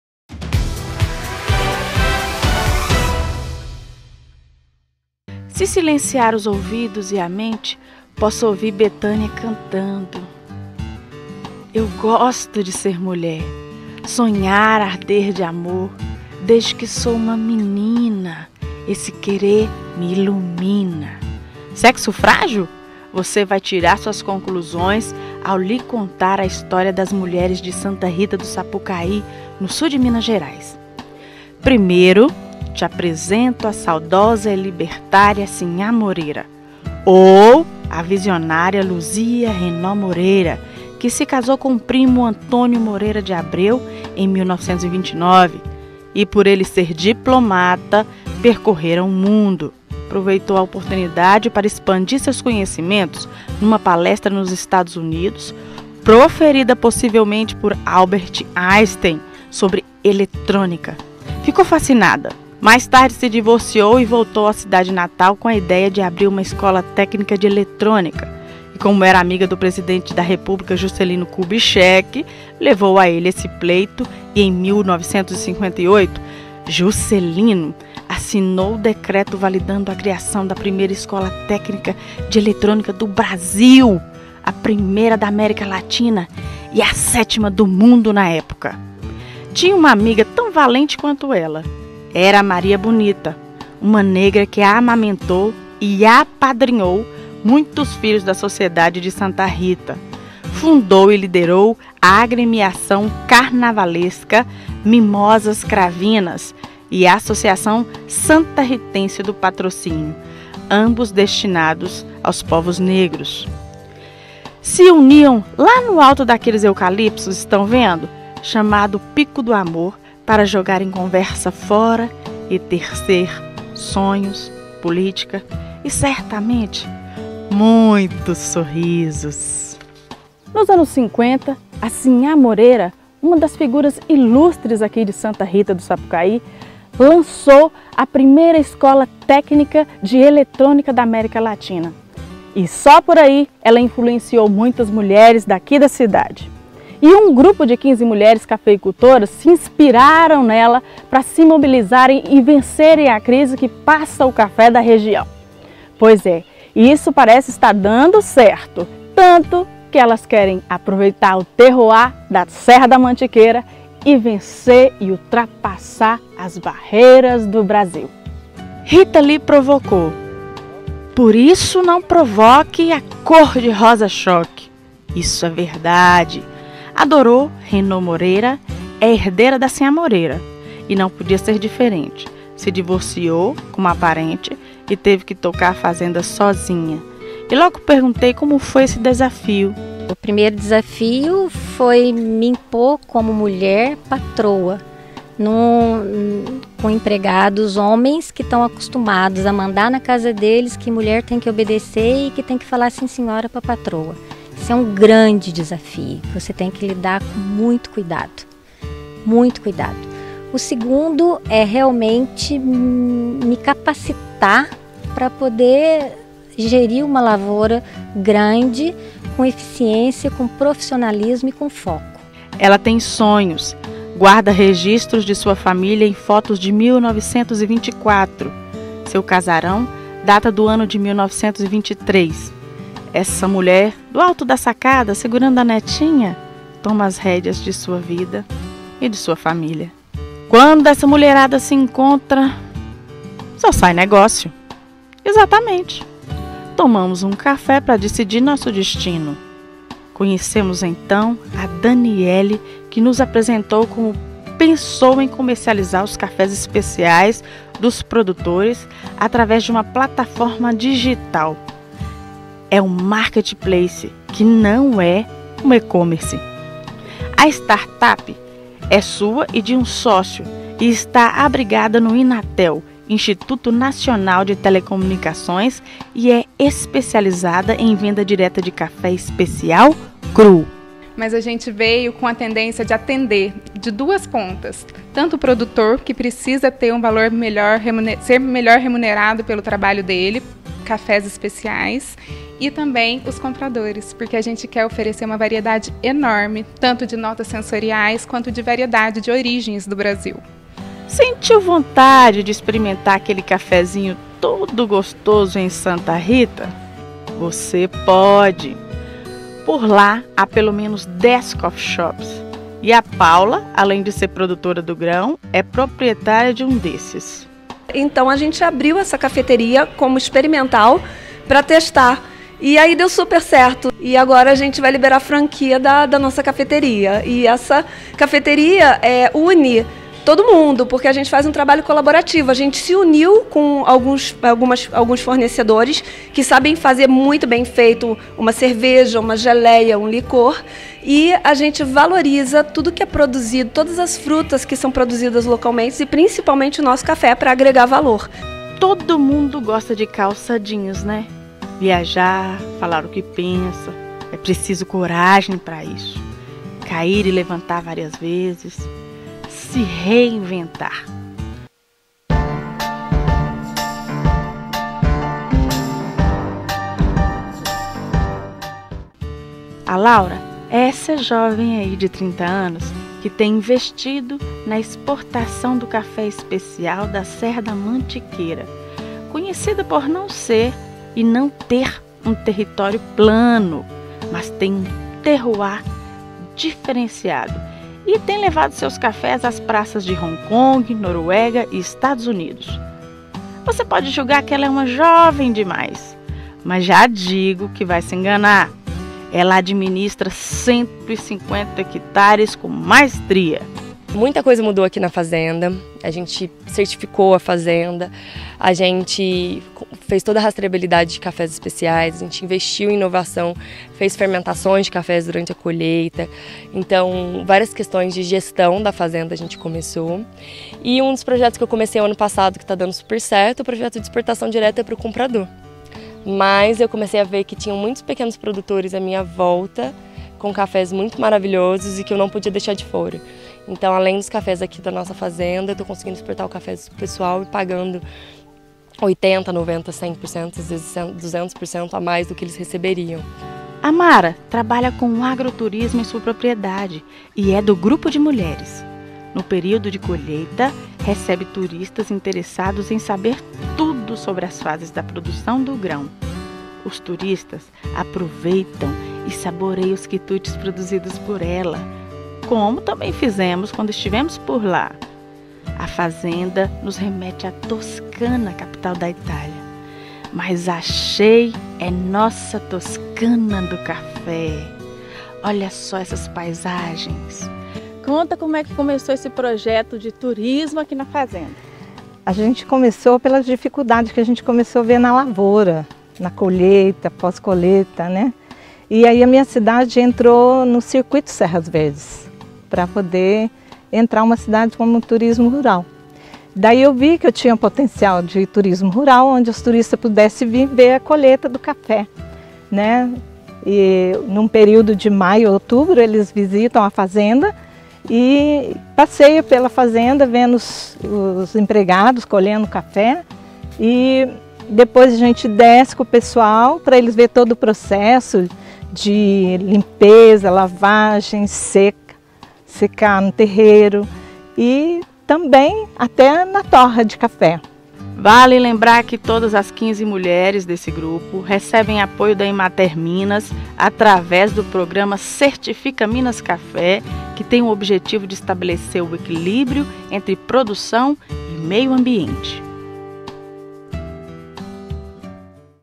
Mulheres que produzem café especial
Mulheres de Santa Rita do Sapucai no Sul de Minas Gerais, são inspiradas em D.Luzia Renno Moreira que na década de 50 teve a ideia de abrir escola técnica eletrônica. Com essa influência as mulheres da cidade se mobilizaram para vencer a crise que passa o café da região. Veja!